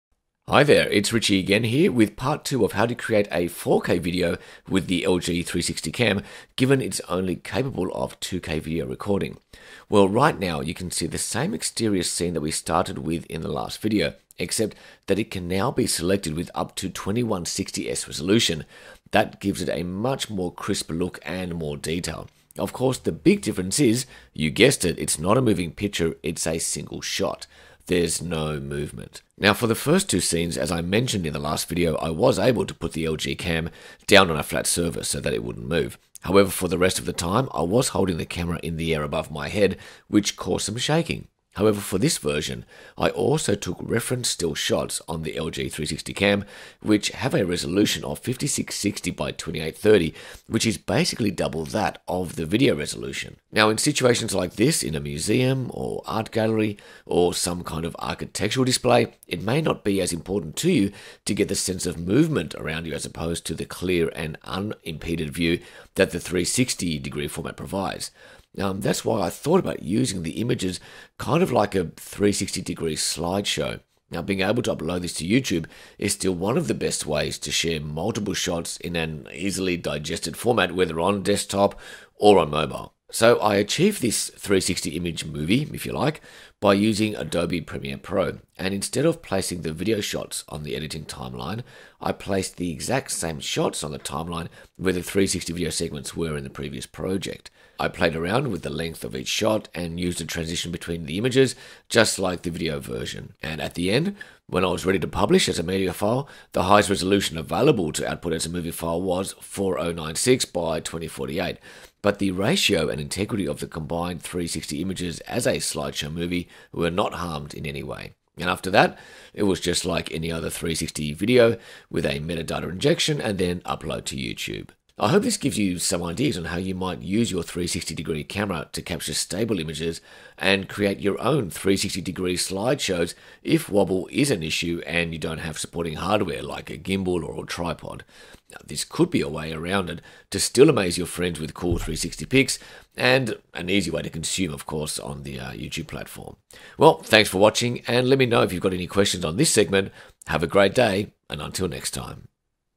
Make 4K 360 Videos With Your LG 360 Cam Part 2
In the second piece of this how-to segment, I show you the same visuals from the previous video, but with an extra 50% resolution - and there's an easy way to achieve it! Watch this video and compare the quality side by side with Part 1 of this segment. Make sure you select the highest possible resolution (should be 4K, 2160s). How do I get  this level of detail with the LG 360 Cam that has 2K video recording? Watch this short video :-)